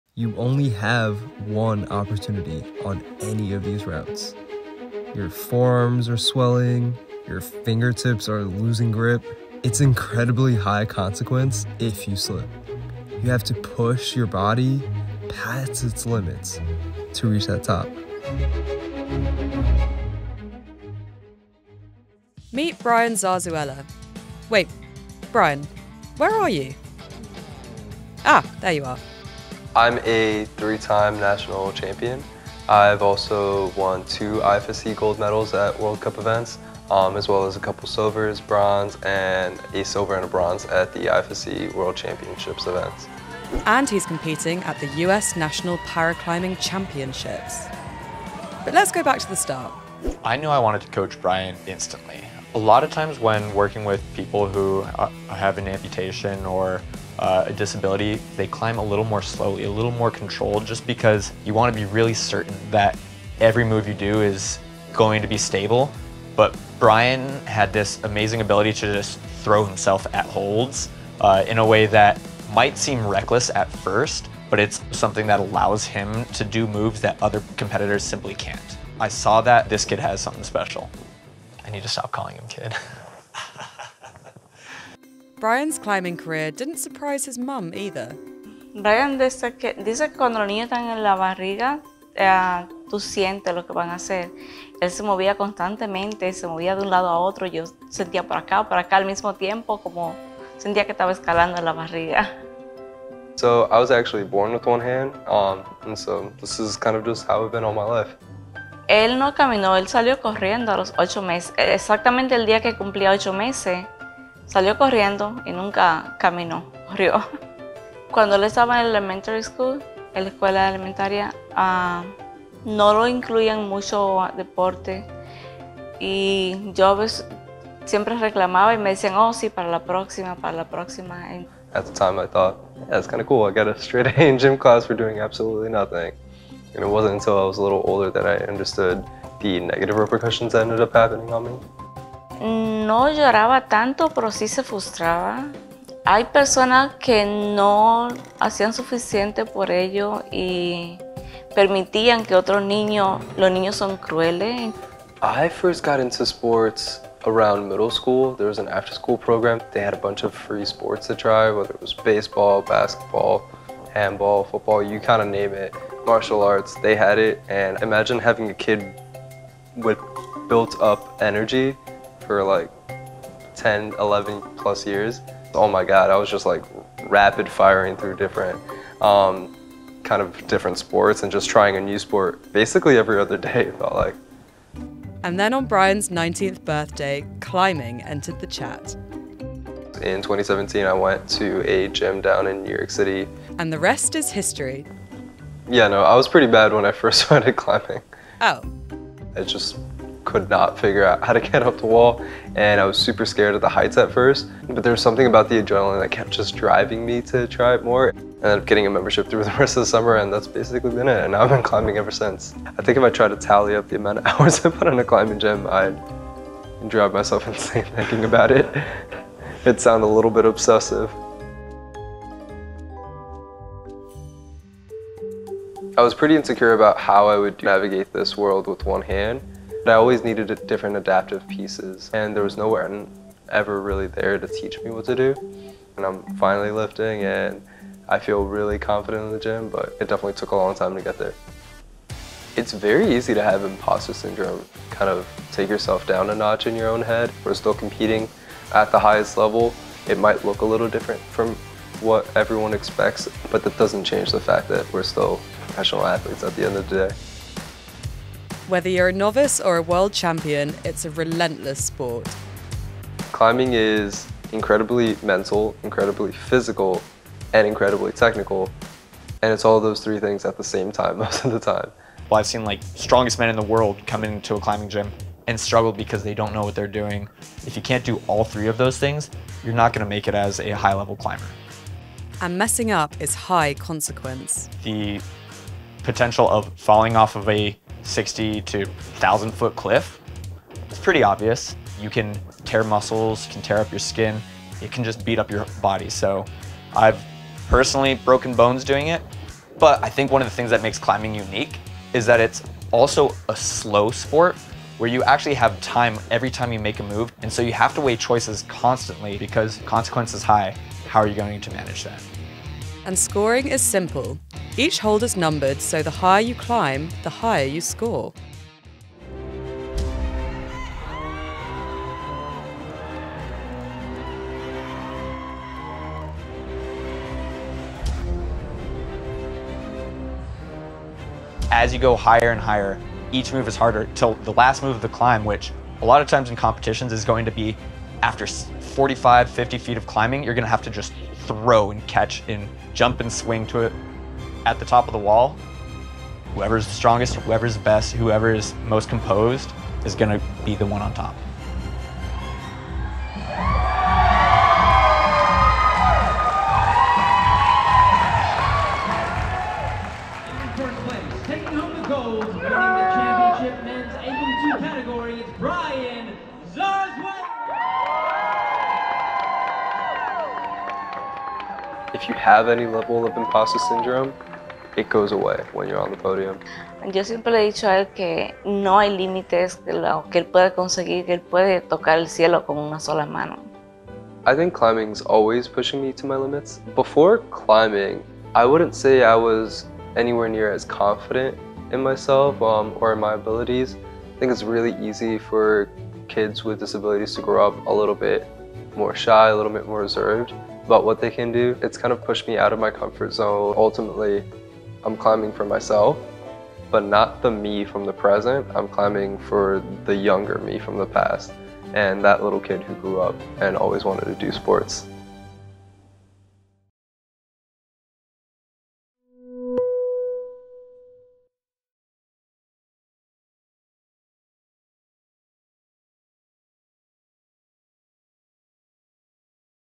Meet the Climbing Champion With One Hand | Super Power
to fuel your own superhero story.

Not many have what it takes to reach the top in climbing. This unforgiving sport is just as much a mental game as it is technical and physical, and has humbled some of the fittest athletes in the world. The wall is however no match for paraclimber Brian Zarzuela.

This athlete doesn't have your average origin story, in fact, Brian was left out of sports as a kid because of his disability. But one day in middle school, he discovered an afterschool club that changed everything. Brian took up baseball, basketball, handball, ice skating, football, BMX, martial arts - you name it he tried it. He'd finally found an outlet for years of built up energy and it just felt right.

Climbing didn't however come naturally. On his 19th birthday, Brian took on his first wall, and he admits, he was pretty bad! But there was something about the sport that kept him coming back. He's winning medals across the world, but how does he perform at the US National Paraclimbing championships?

This Great Big Story is powered by @HuelYT

00:00 Meet Brian Zarzuela
01:25 Growing up
02:35 Finding sports
03:45 Overcoming insecurities
04:18 Why an extreme sport?
05:19 Competition Day
06:00 The Results
06:17 Outro

Lead Producer - Annabel Fraser
Camera - Rafael Abreu Canedu
Editor - James Rayner
Graphics - Will Minns

Got a story idea for us? Shoot us an email at pitch [at] GreatBigStory [dot] com 📧